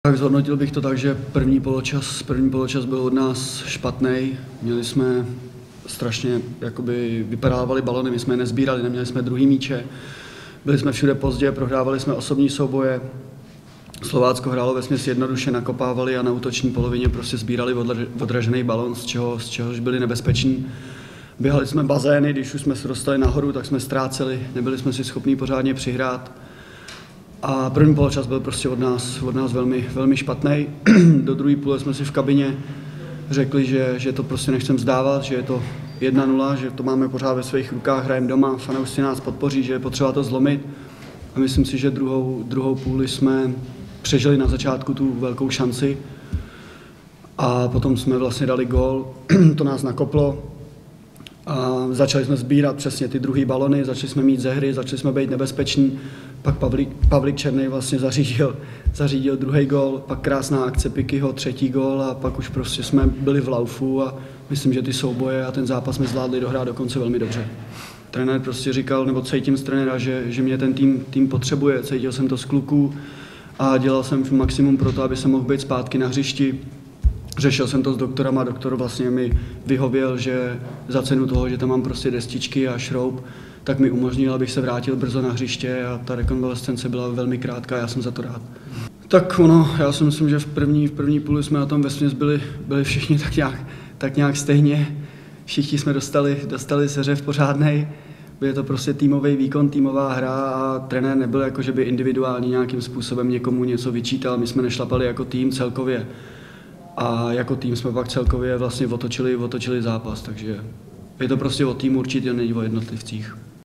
Dominik Kostka na pozápasové tiskové konferenci
Jedním z hrdinů výhry nad Slováckem byl v utkání 29. kola FORTUNA:LIGY záložník Dominik Kostka. Co krátce říkal na pozápasové tiskovce?